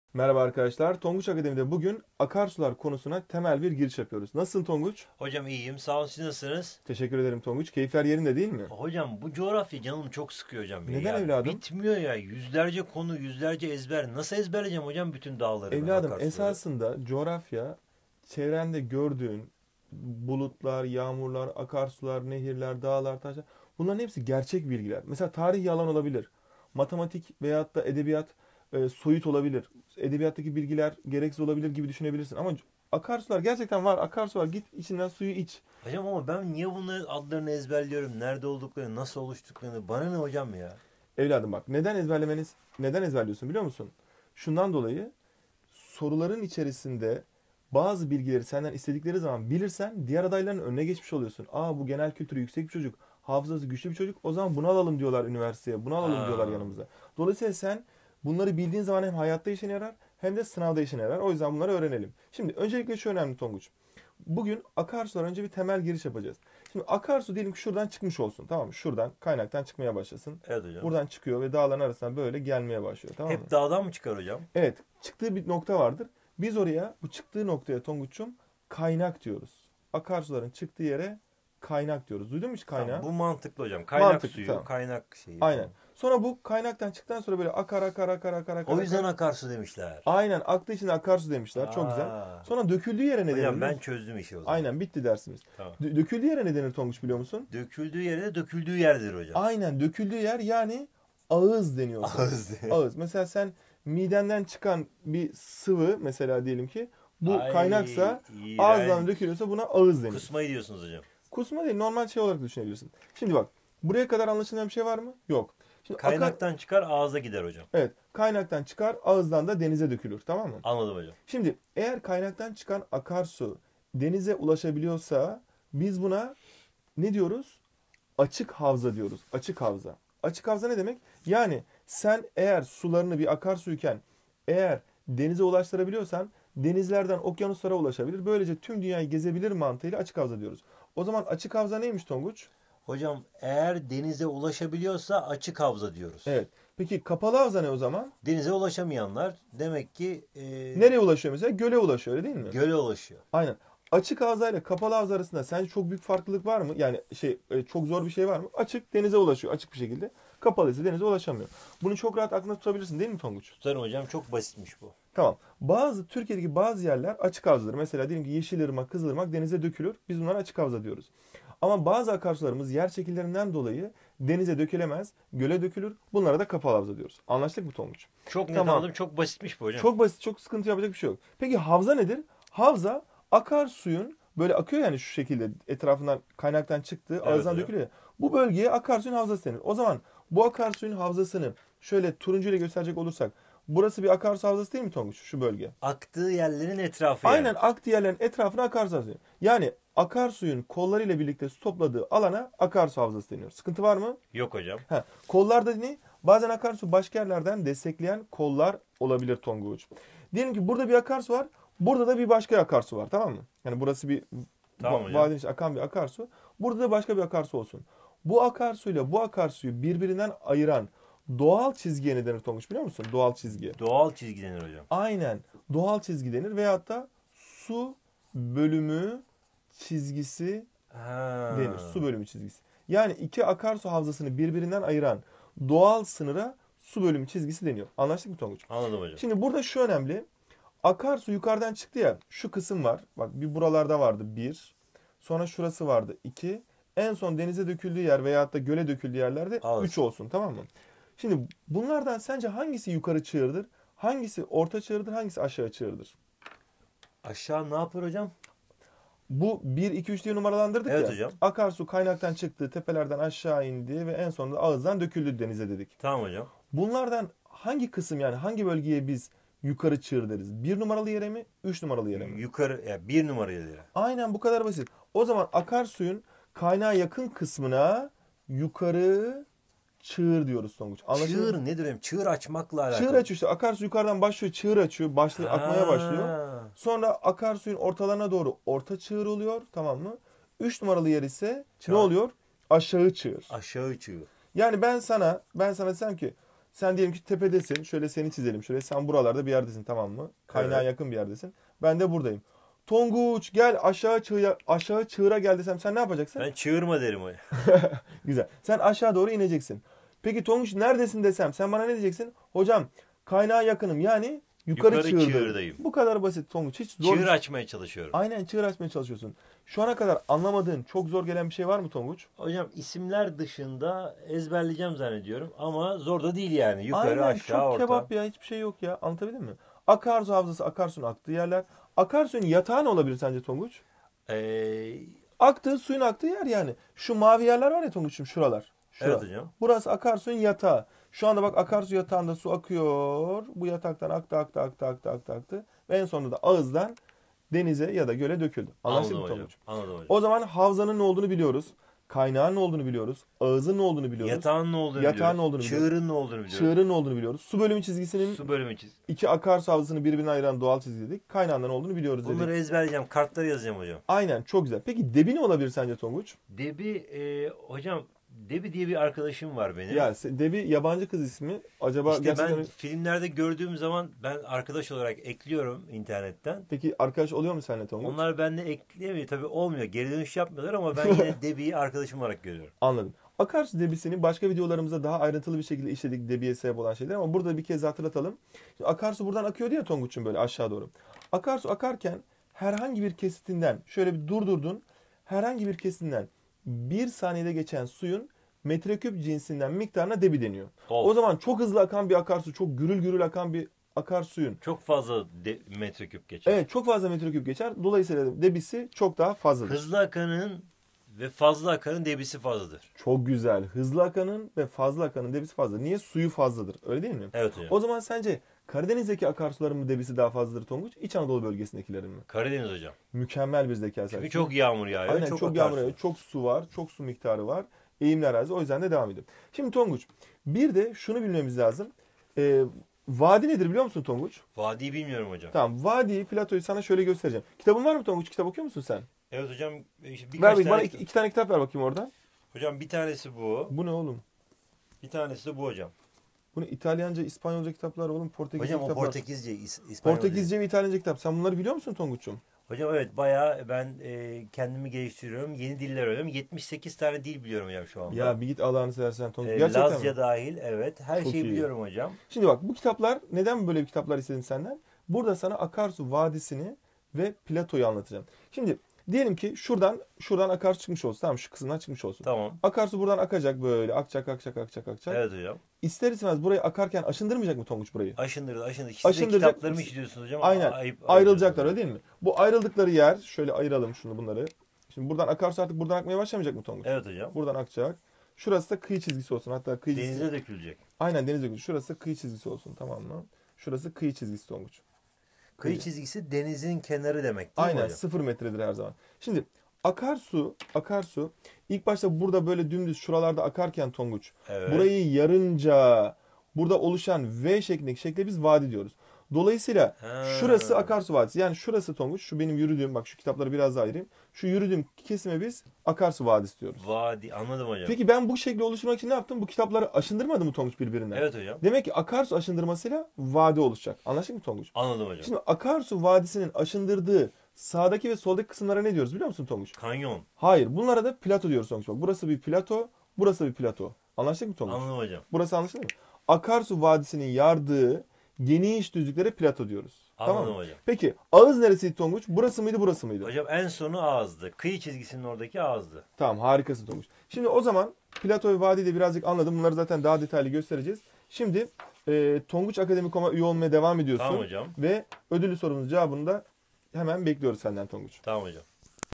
9dk'da AKARSULAR
9dk'da AKARSULAR konusunu kısa ve kolay anlaşılan bir anlatımla öğrenmek istemez misin? Çıkabilecek soruların özellikle altını çizdiğimiz bu videoyu sakın kaçırma! Tonguç bile yapıyorsa sen daha iyisini yapabilirsin!
Ayrıca paylaşmak güzeldir. Bizi Instagram, Facebook ve Twitter'dan da takip et.